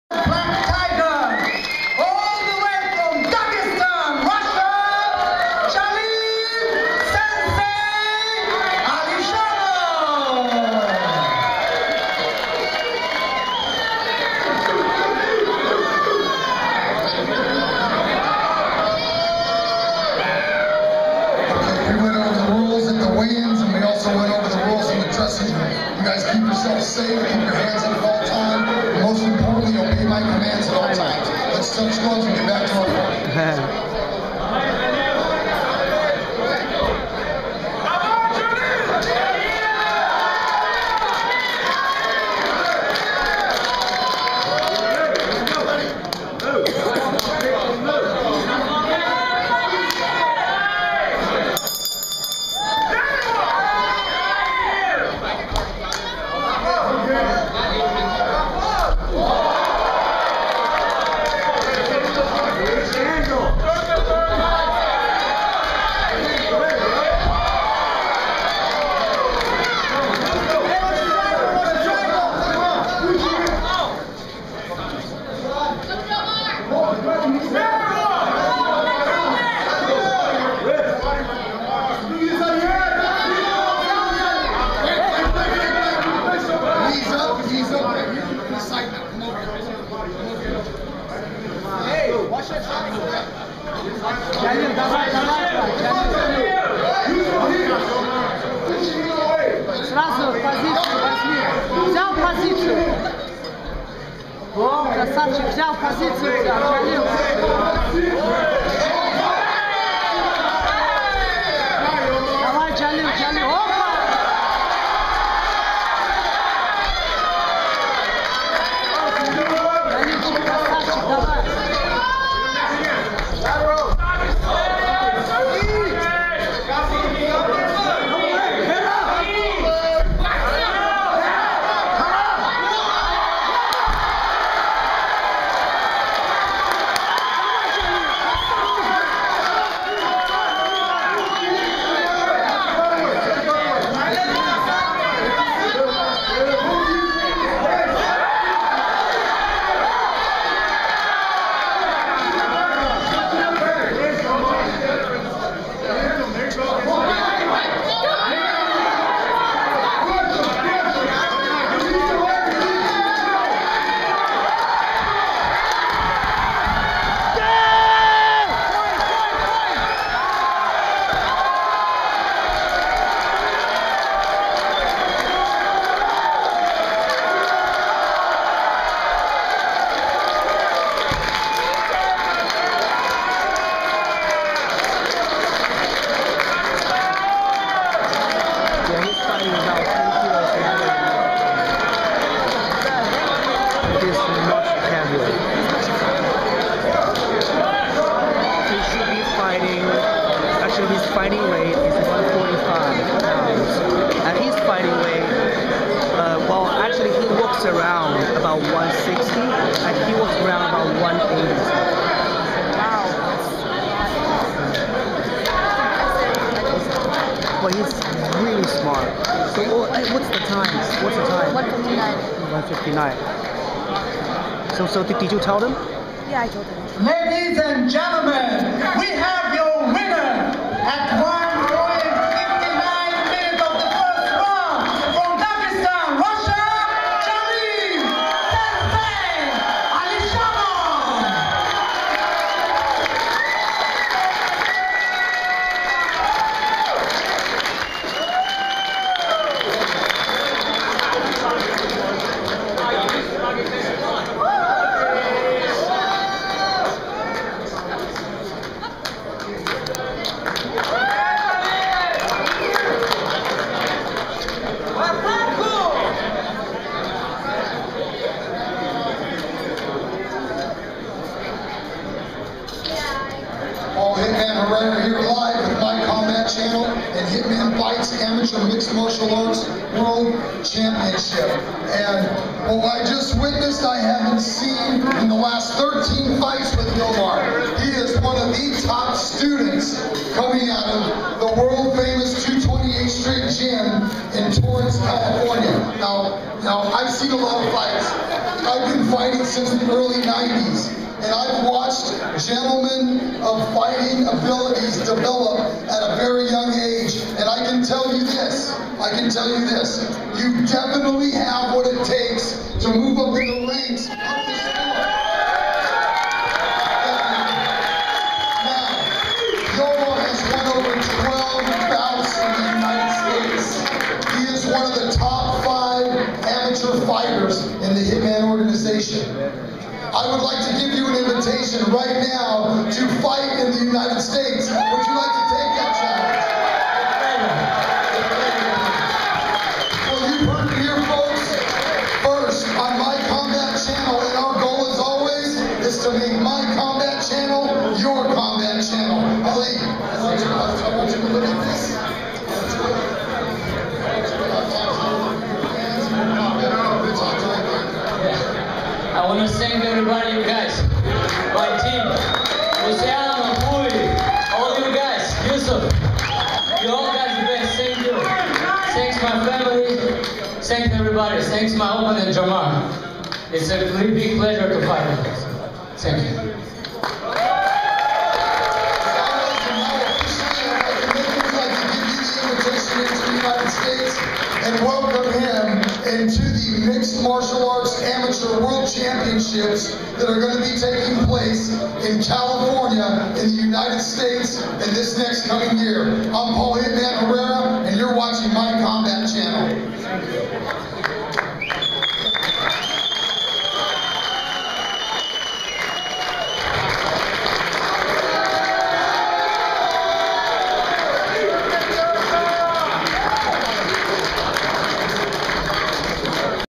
Jalil Alizhanov vs.Jomar Vendrell
Dzhalil Alizhanov vs.Jomar Vendrell, Nov.19, 2011, Hilton Hotel, Beijing, China